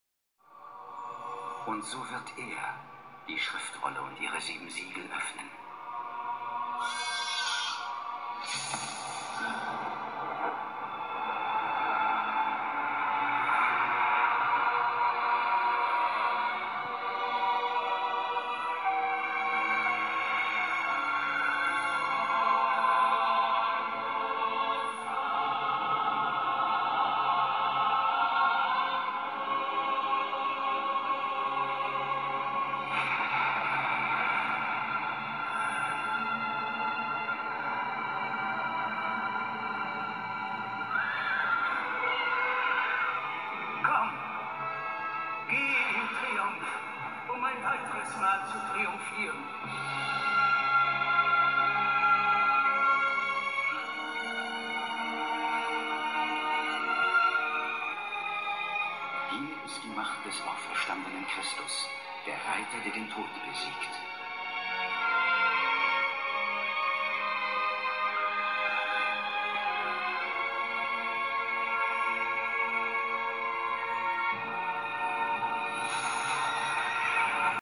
لان المسيح كسر شوكة الموت بالفداء وأهدانا الحياةالابدية (رويا يوحنا الرسول)
المسيح هو الحمل الوديع الذي هزم الموت بالفداء.روى يوحنا الرسول.

Denn Christus hat den Don des Todes zerbrochen und ist auferstanden und hat uns das ewige leben geschenkt ,ist er allein würdig.Amen
المسيح هو الفارس الذي هزم الموت. امين.